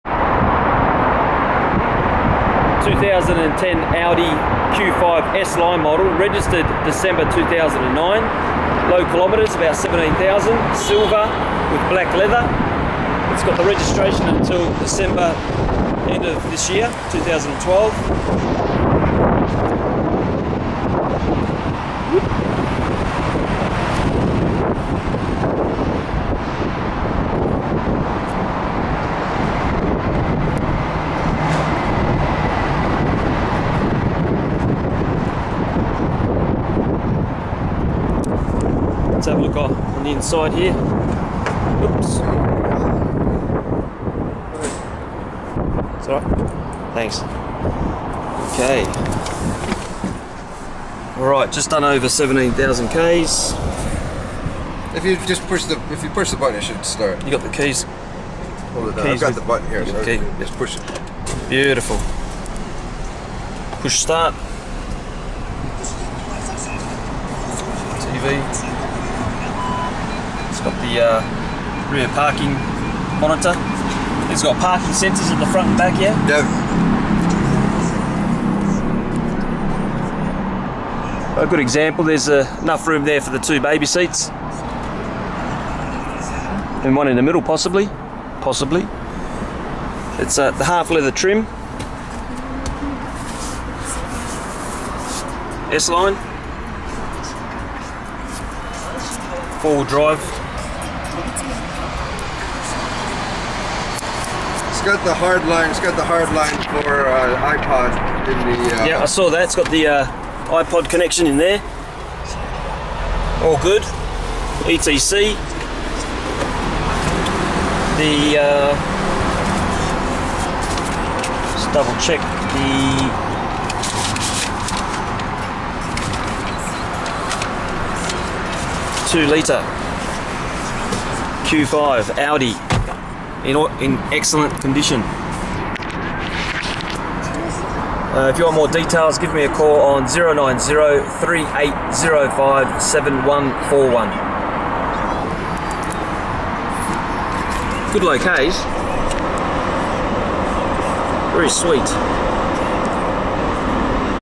2010 (first reg Dec 2009) Model Audi Q5 2.0T Sline - For Sale Tokyo Japan
Call Mick Lay direct - Tokyo Japan on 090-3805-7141
Great condition with low km (about 17,000 km) and shake (car registration) to Dec 2012.  Right hand drive, silver color with black 1/2 leather interior seats, original navigation with rear parking monitor... Call to check this vehicle out (this is a private sale).   This car is presently (May 2012) retailing for about 5 million yen plus taxes.  Good chance to get a great deal.